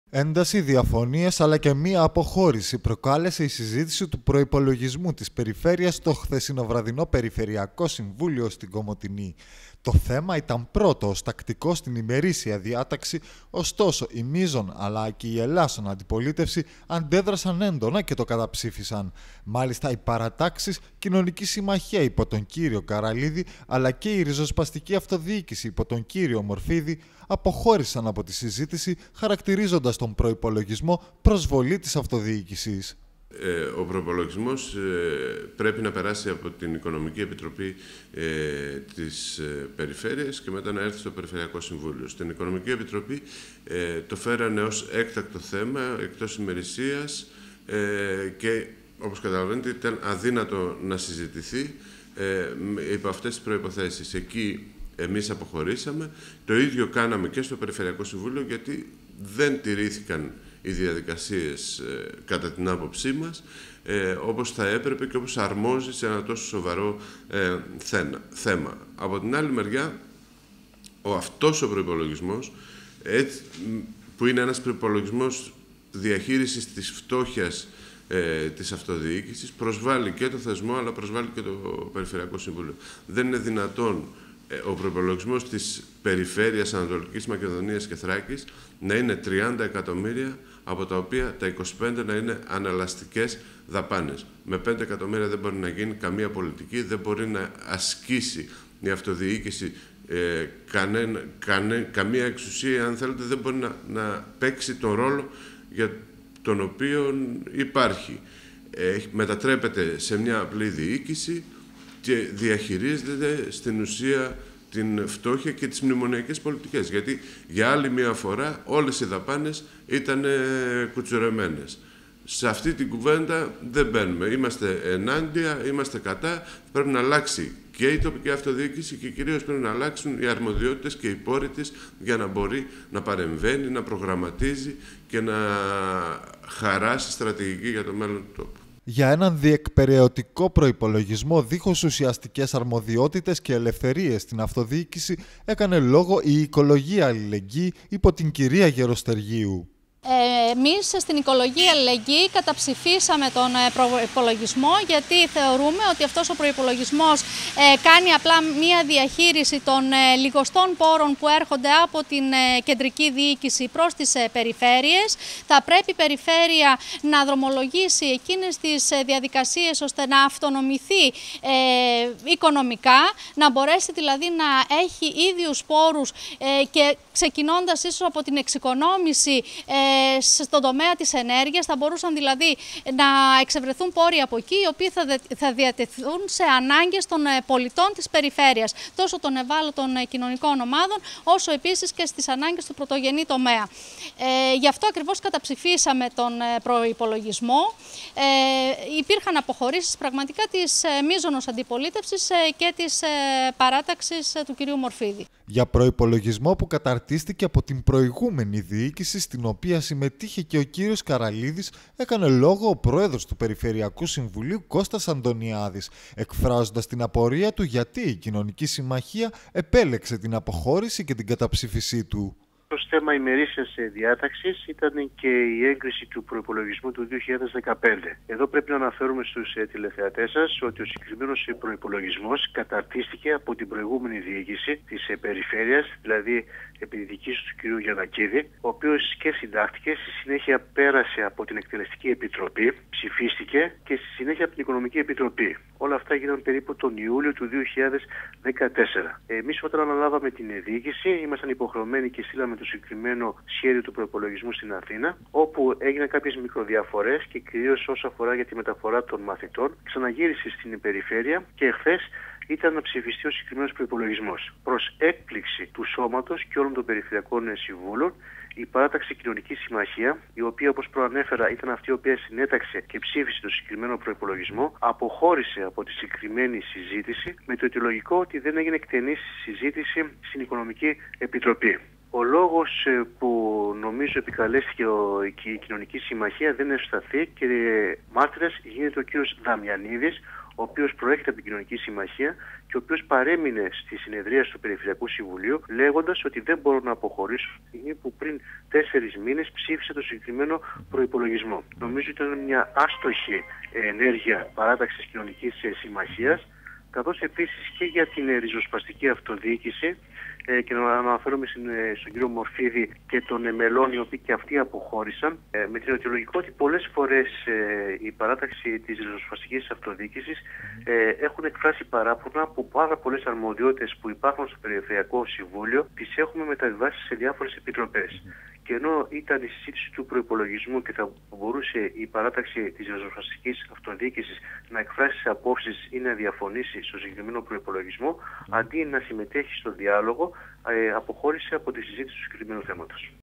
Ένταση στον προυπολογισμό του Περιφερειακού Συμβουλίου
Ένταση διαφωνίες αλλά και μία αποχώρηση προκάλεσε η συζήτηση του προυπολογισμού της Περιφέρειας στο χθεσινοβραδυνό Περιφερειακό Συμβούλιο στην Κομοτηνή. Το θέμα ήταν πρώτο ως τακτικό στην ημερίσια διάταξη ωστόσο η μείζονα αλλά και η ελλάσονα αντιπολίτευση αντέδρασαν έντονα και το καταψήφισαν. Μάλιστα οι παράταξεις Κοινωνική Συμμαχία υπό τον κ. Καραλίδη αλλά και η Ριζοσπαστική Αυτοδιοίκηση υπό τον κ. Μορφίδη αποχώρησαν από την συζήτηση χαρακτηρίζοντας τον προυπολογισμό προσβολή της αυτοδιοίκησης.

Για έναν διεκπαιρεωτικό προυπολογισμό δίχως ουσιαστικές αρμοδιότητες και ελευθερίες στην αυτοδιοίκηση έκανε λόγο η Οικολογία Αλληλεγγύη υπό την κ. Γεροστεργίου.

Για προυπολογισμό που καταρτίστηκε από την προηγούμενη διοίκηση στην οποία συμμετείχε και ο κ. Καραλίδης έκανε λόγο ο πρόεδρος του Περιφερειακού Συμβουλίου Κώστας Αντωνιάδης εκφράζοντας την απορία του γιατί η Κοινωνική Συμμαχία επέλεξε την αποχώρηση και την καταψήφιση του.